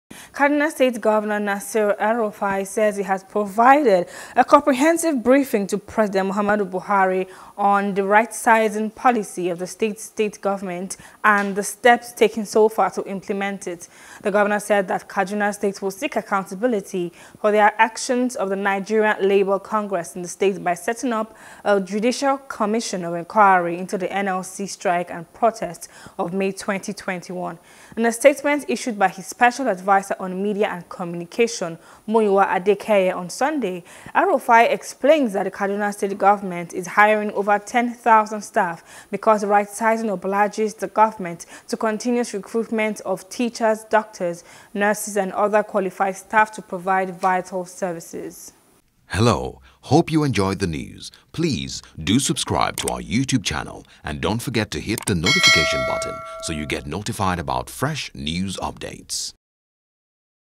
NLC: No Going Back On Sack Of Civil Servants, El-Rufai Tells Buhari | NEWS
Kaduna State Governor Nasir El-Rufai says he has provided a comprehensive briefing to President Muhammadu Buhari on the rightsizing policy of the state State Government and the steps taken so far to implement it.